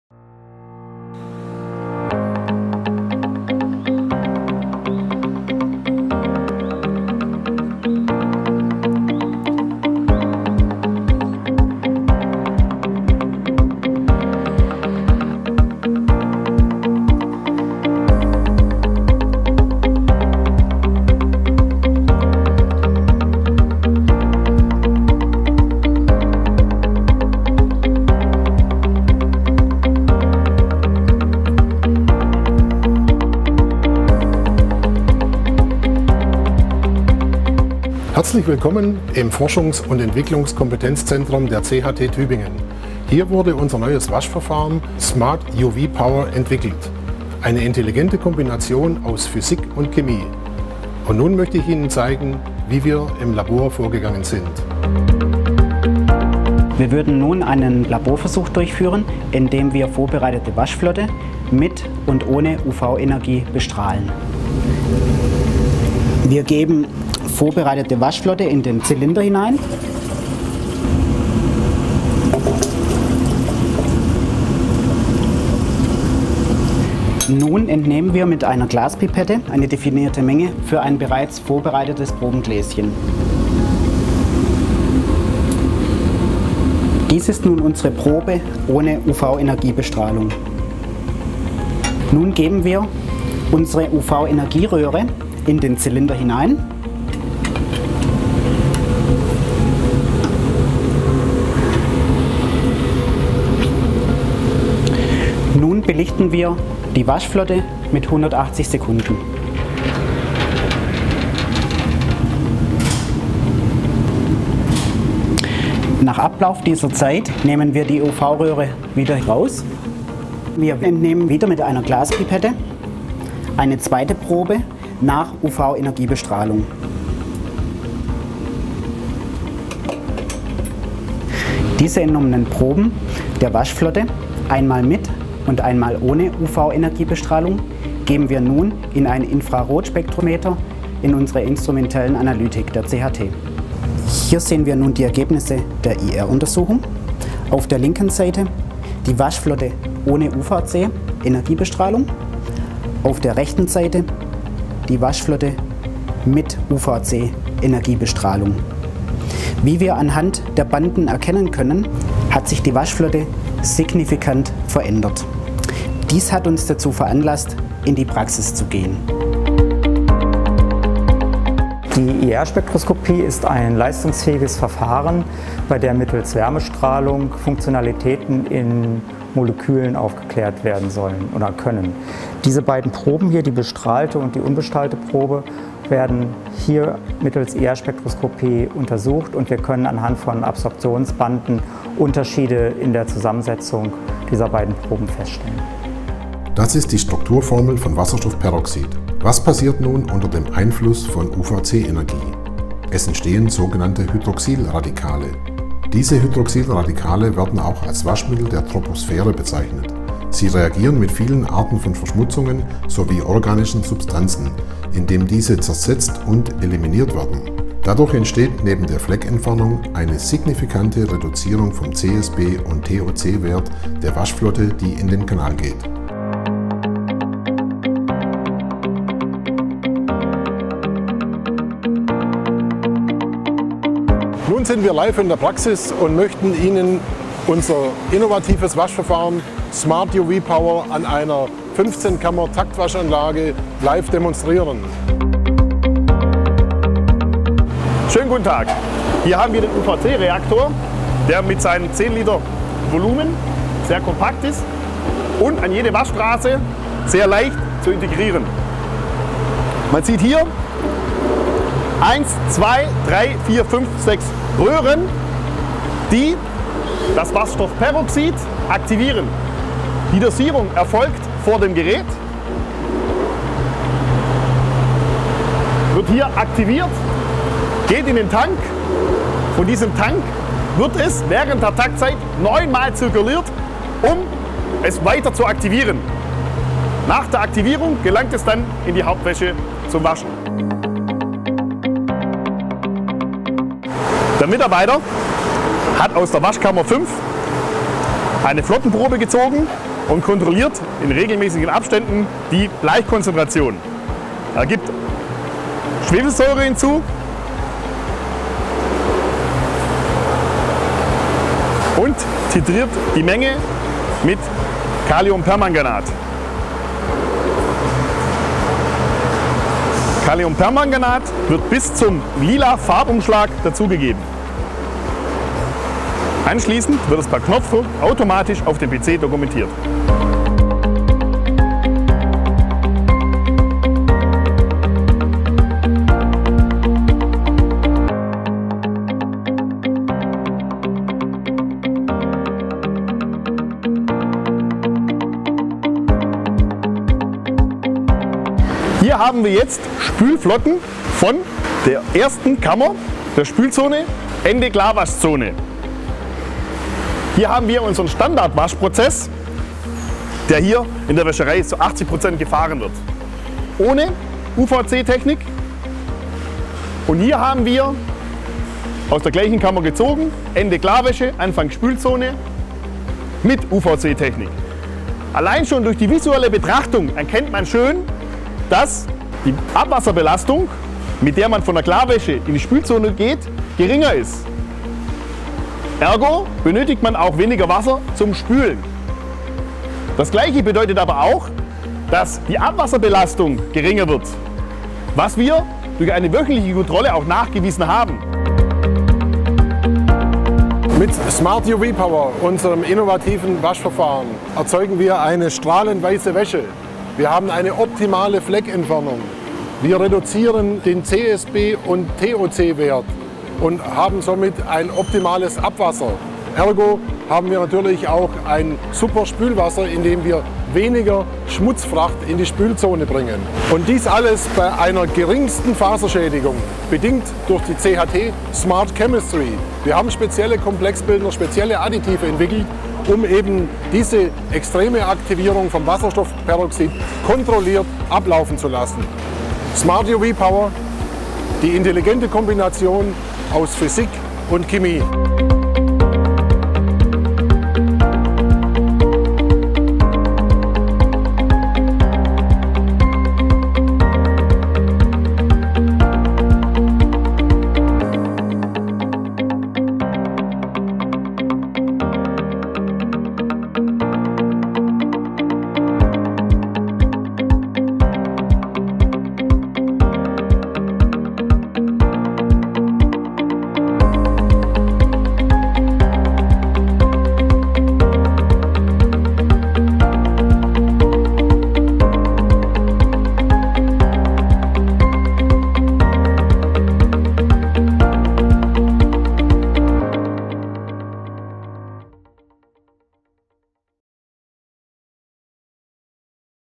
CHT SMART UV POWER Waschverfahren
Mit SMART UV POWER der CHT Gruppe lassen sich Waschprozesse entscheidend optimieren, um wertvolle Ressourcen einzusparen und die Umweltbelastung zu reduzieren. Die intelligente Kombination aus Chemie und Physik eröffnet eine völlig neue Dimension für den gesamten industriellen Waschprozess.